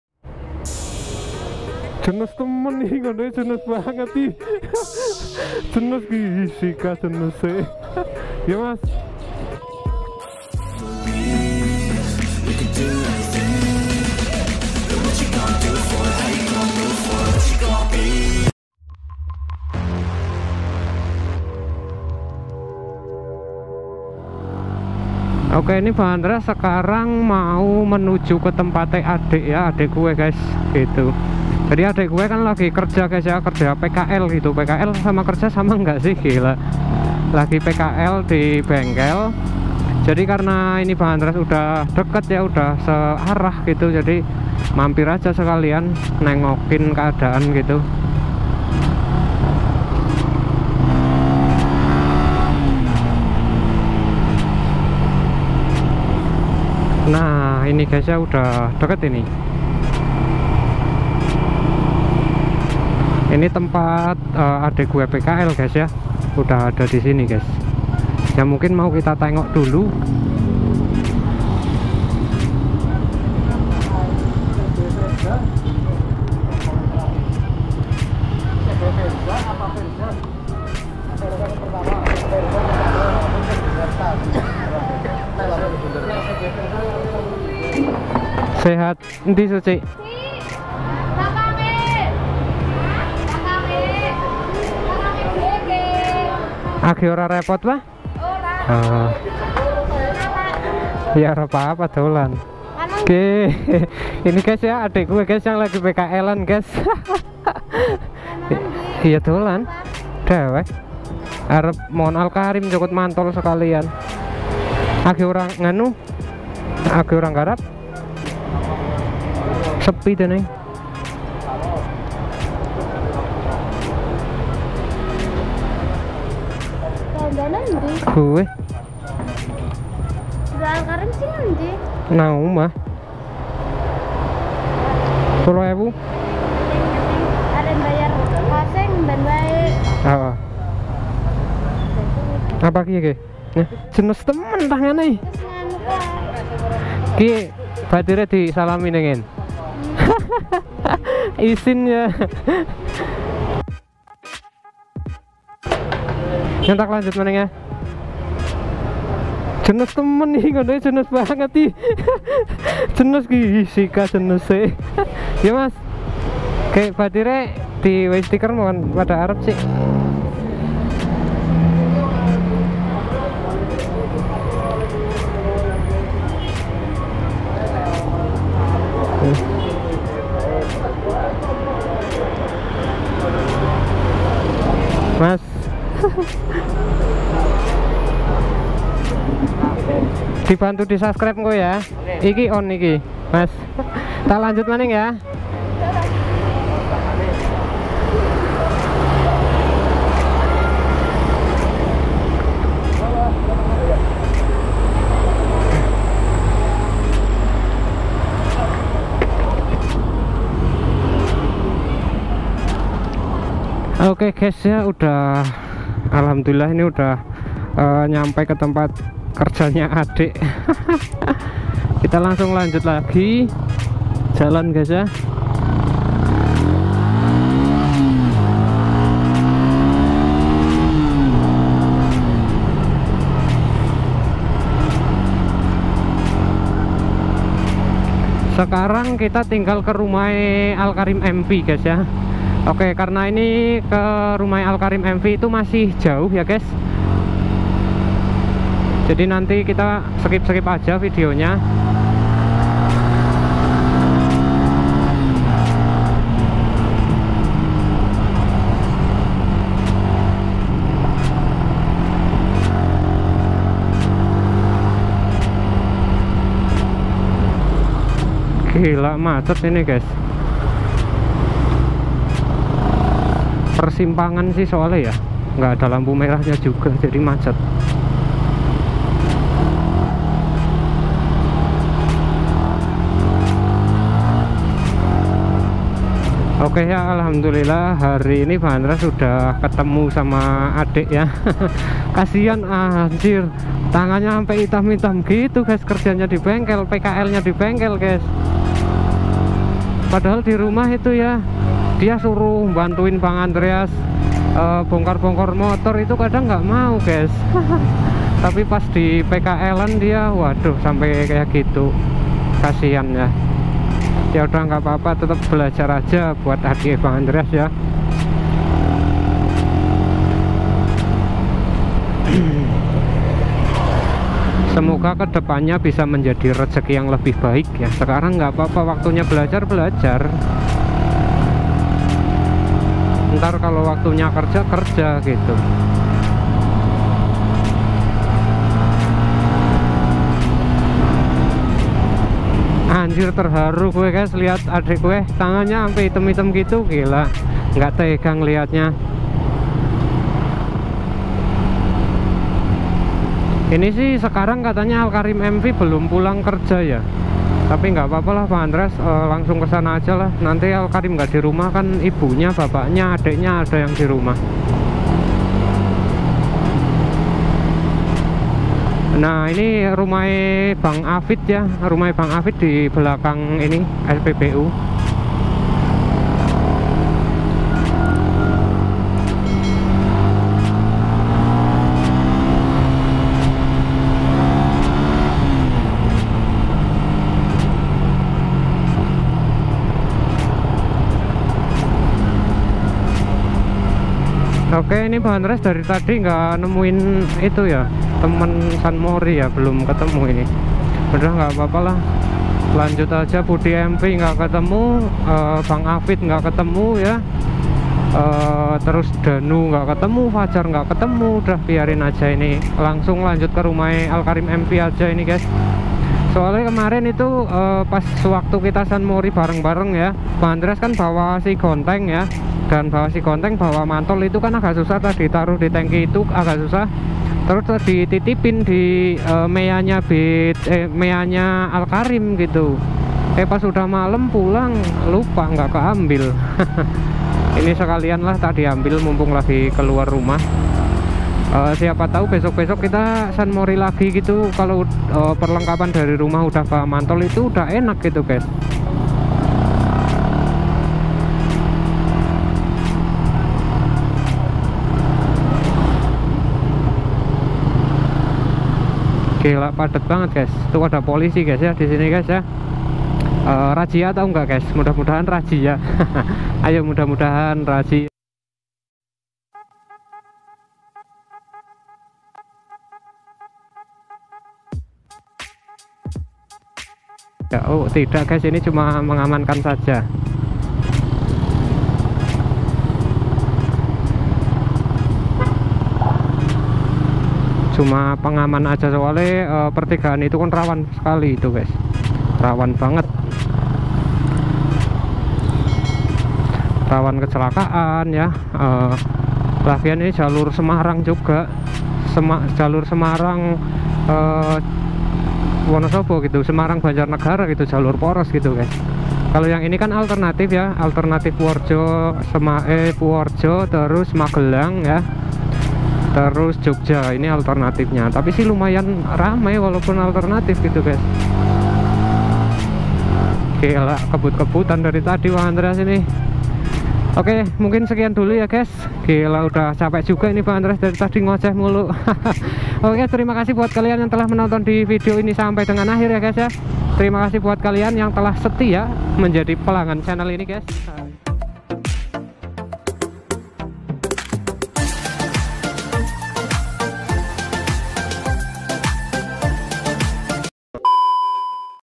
datang ke bengkel demi seorang gadis, dua gadis manis penghuni bengkel
kita lanjut vidionya yaitu kita otw ke bengkel langganan sekalian jenguk adik sy yang lagi PKL di bengkel tersebut.

Terimakasih buat kalian yang sudah menonton dan jangan lupa bantu SUBCRIB YA 🙏🙏